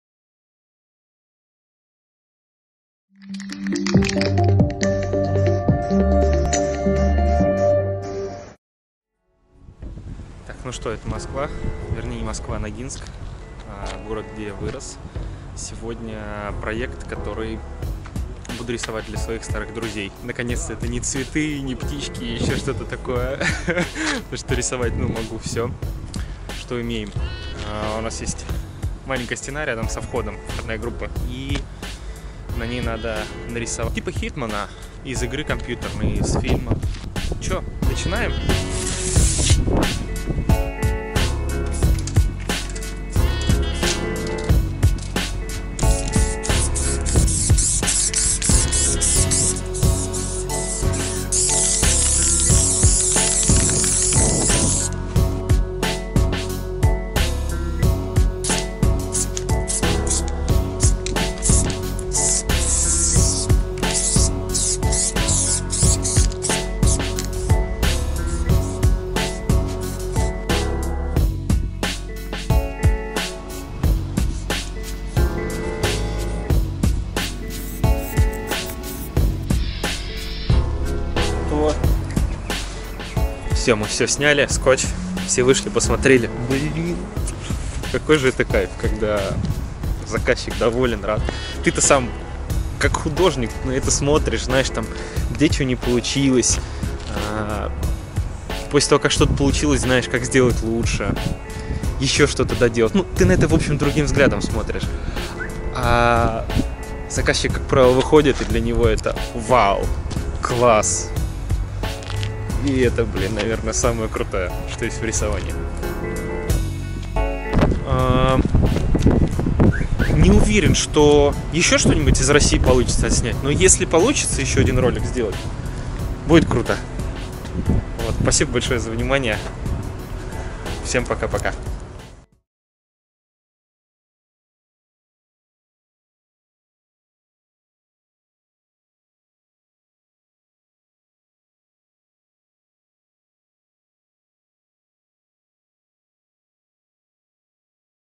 GRAFFITI MURAL with HITMAN FACE. HOW TO PAINT A FACE. GZART
This mural is my last mural in Moscow before i came back at NYC.
For couple hours i paint a hitman face with out any projectorsor or  tracer lines. And u can see how i paint a face with spray.
Few colours of spray paint, few hours of free time and mural is done.
This face more like character than fotorealistic but still looks fine and was made very quick